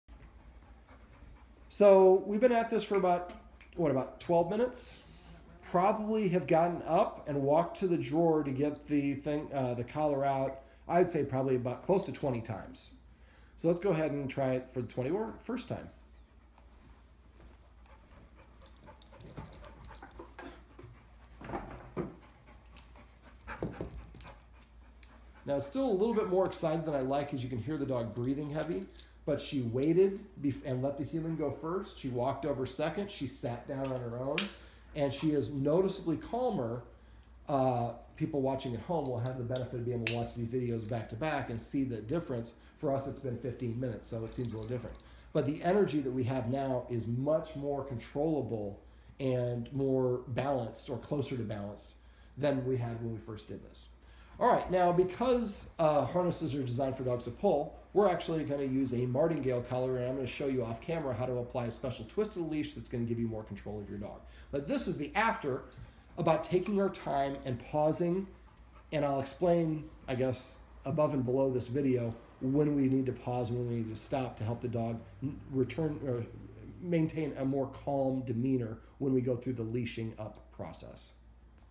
Getting Aspen Ready for a Walk - After
Aspen is a five-year-old Pit Bull mix who lives with thirteen-year-old Tiger, also a Pit mix. The guardians set up this dog obedience training session with me to primarily work with Aspen on his dog aggression, excitement and accidents in the house.

This is one of the videos we shot during their session.